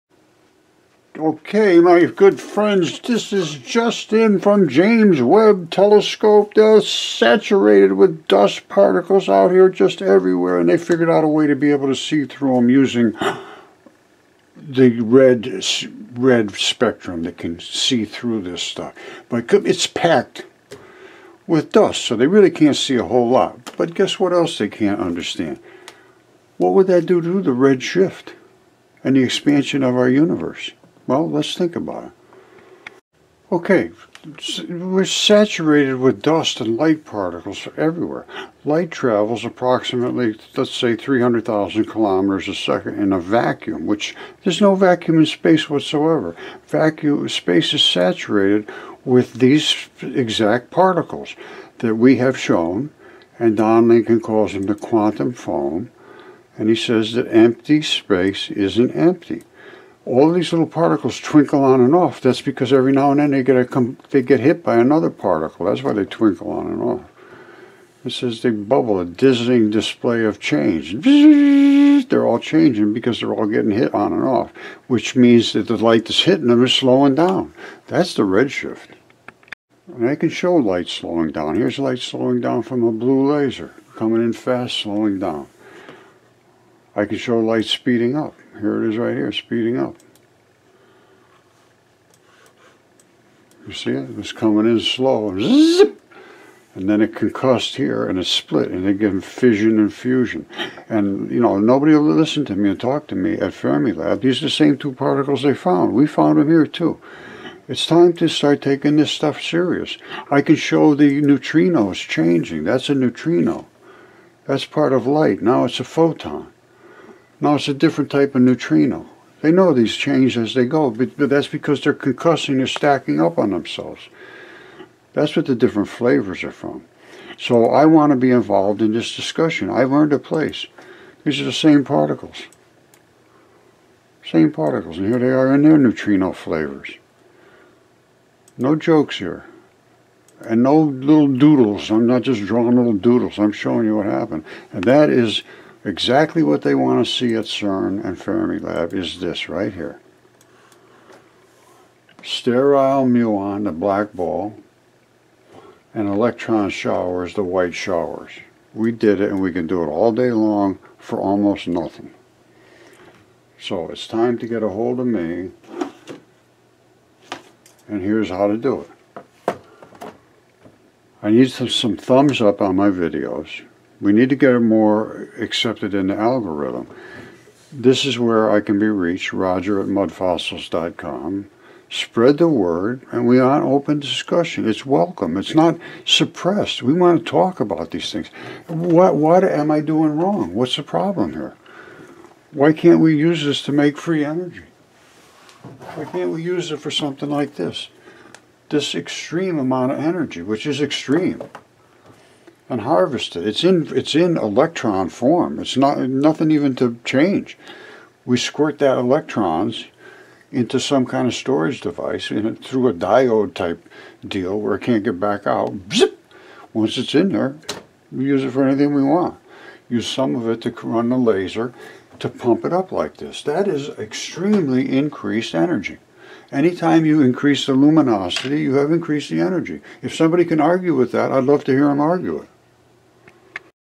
Galactic Dust Found Everywhere and it Slows light down so Universe Expansion is not likely
Red Shift means light is slowing down and that does not happen in a COMPLETE vacuum but Open space is saturated with dust and other particles. According to Fermilab   "Empty Space is not Empty".
Latest research shows space is literally saturated with particles. Everything that Hubble is based on RELIES on a VACUUM which simply does not exist.
The Expansion of the Universe is in doubt..time for all new Science.